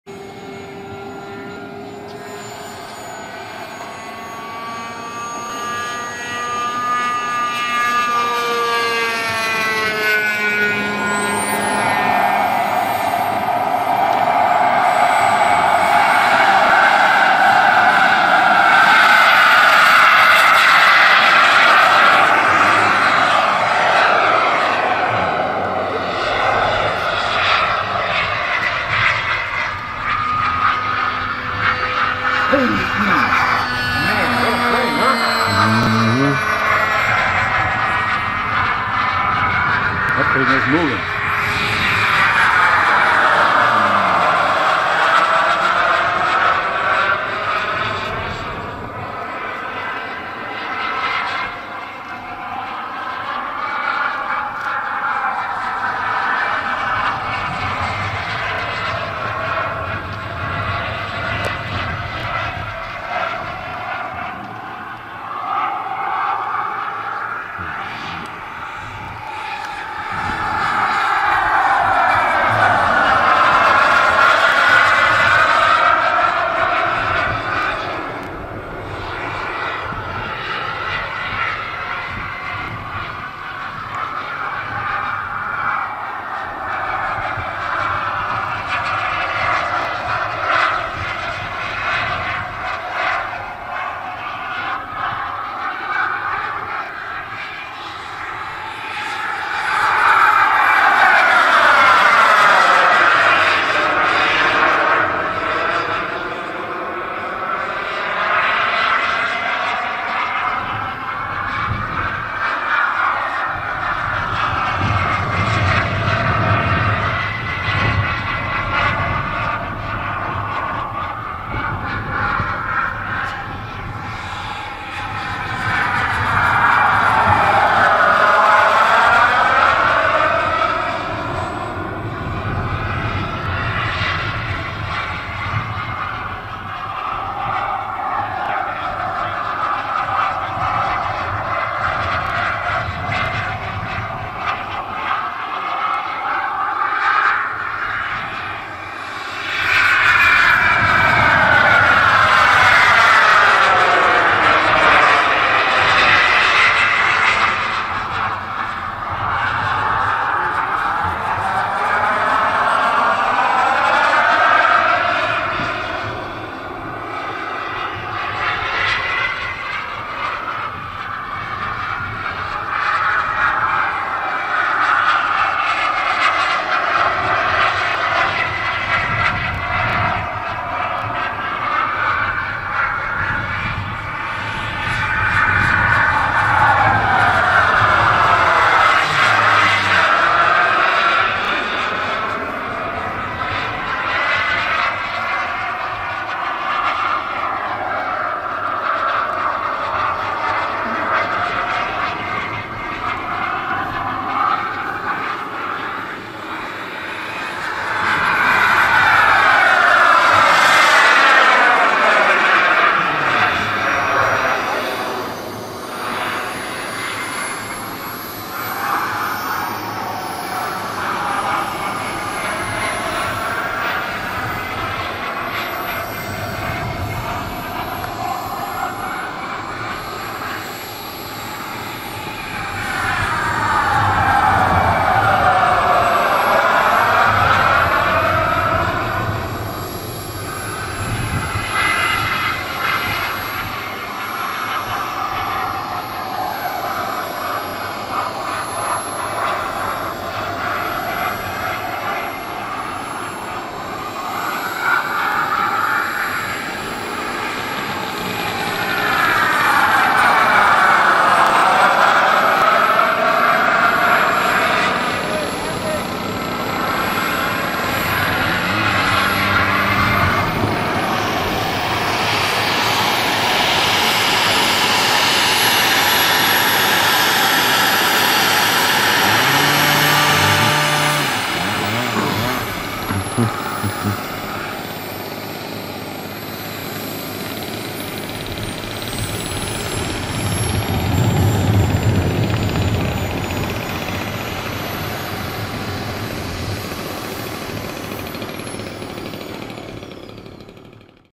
YA F-15 Eagle
Yellow Aircraft F-15 Eagle being flown by Jerry Keller.